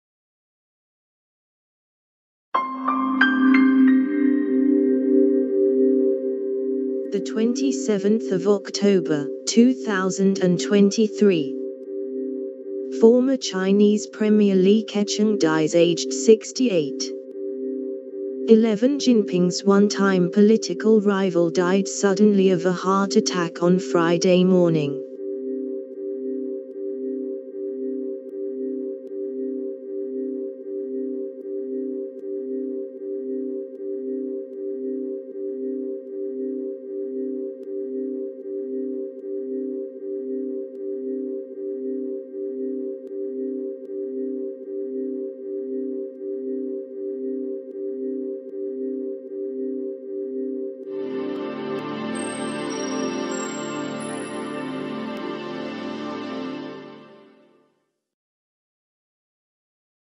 [Latest]Former Chinese premier Li Keqiang dies aged 68
[Contents]
Xi Jinping’s one-time political rival died suddenly of a heart attack on Friday morning.